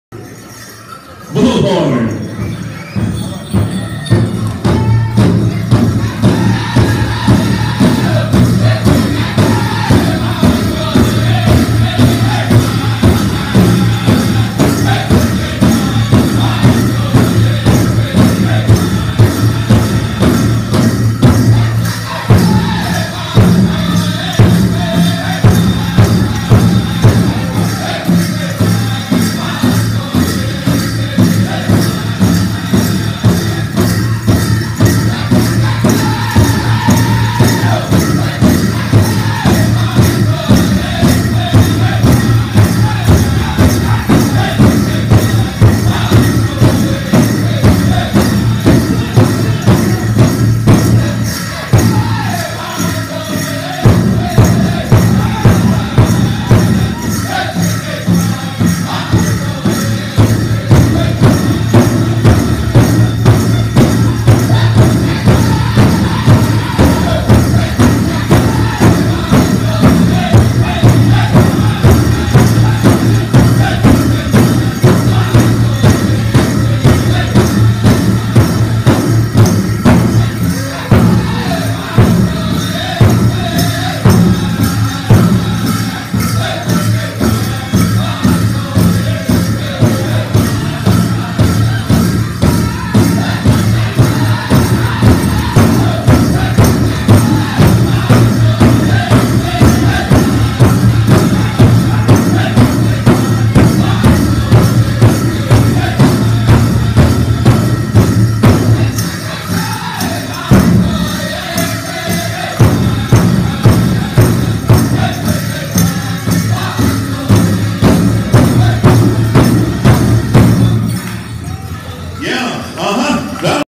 Men's Grass...Arrow Lakes Powwow 2016
Men's Grass at Arrow Lakes powwow in Inchelium, WA, song number 2